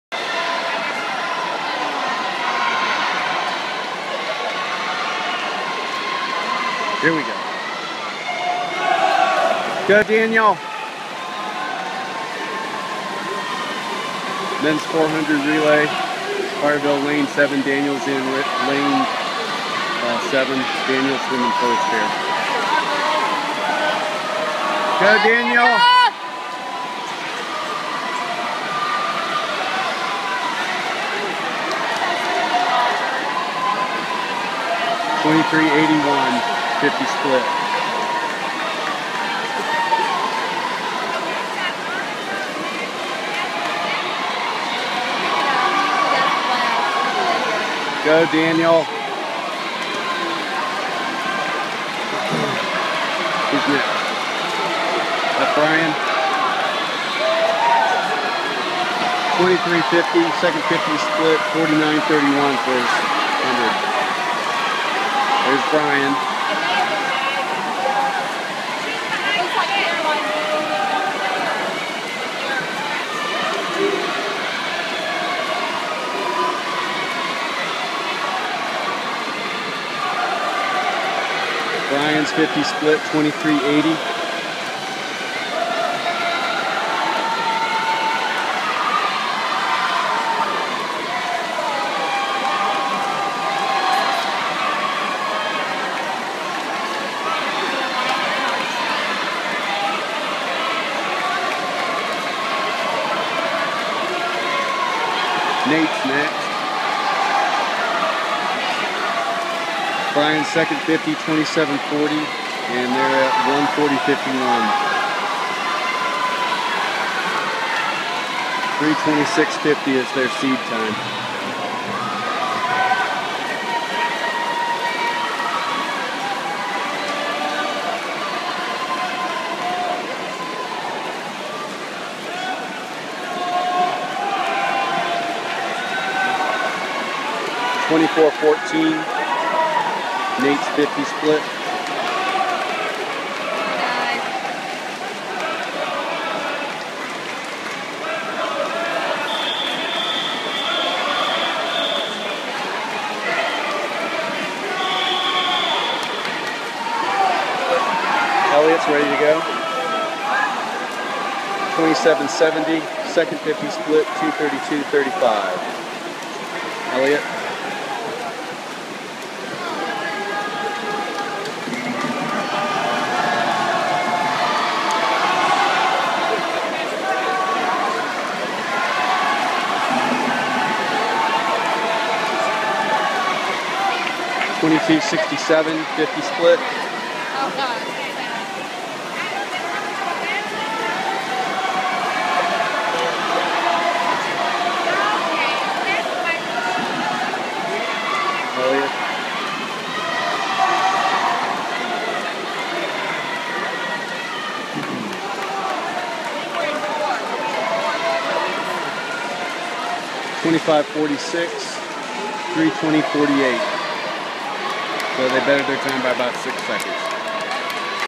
TN State HS Swim Meet - 400 Free Relay
CHS in lane 7.
Daniel Hayes
Brian Fay
Nate Briant
Elliott VanDeren